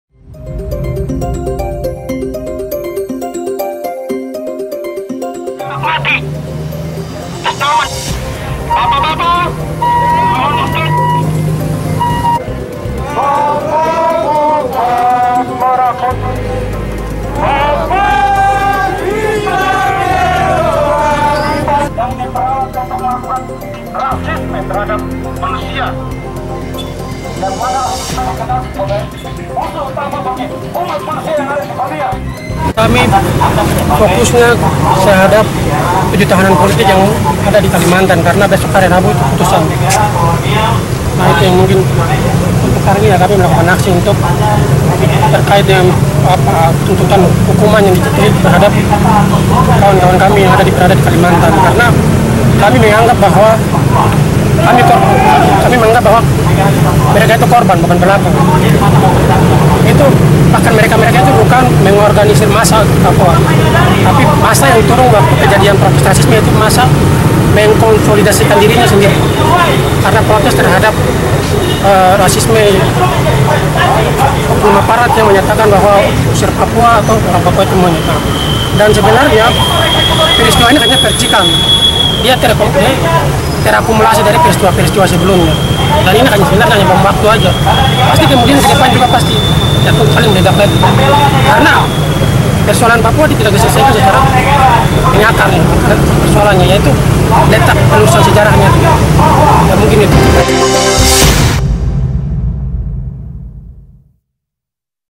Aksi Unjuk Rasa Tuntut Pembebasan 7 Orang Aktivis Papua
SURABAYA, KOMPASTV  Puluhan massa dari Aliansi Mahasiswa Papua melakukan aksi unjuk rasa di depan gedung Negara Grahadi Surabaya untuk menuntuk agar 7 aktivis Papua yang ditahan di Balikpapan dibebaskan.

Dengan membawa poster bergambar foto 7 aktivis Papua massa menuntut agar 7 aktivis Papua yang diduga memicu kerusuhan di Papua pada 2019 lalu untuk dibebaskan tanpa syarat.

Mereka menganggap penangkapan tersebut merupakan sebagai tahanan politik dan mengecam proses persidangan maupun penahanan terhadap rekannya tersebut.

Koordinator lapangan aksi demo Yoab Orlando sebutkan 7 tahanan politik tersebut merupakan korban.

"kami fokusnya terhadap 7 tahanan politik di Kalimantan yang besok putusan terkait tuntutan hukum kawan kami di kalimantan, kami menganggap mereka korban, mereka bukan mengorganisir massa tapi massa yang mengkonsolidasikan dirinya karena protes terjadinya rasisme," Ujar Yoab Orlando Korlab

Namun ironis, aksi unjukrasa tersebut diselipi dengan aksi separatisme dengan membentangkan poster bergambar bendera bintang kejora dan menyanyikan lagu makar.

Meski begitu aksi tersebut berlangsung damai karena mendapat penjagaan ketat aparat kepolisian Polrestabes Surabaya.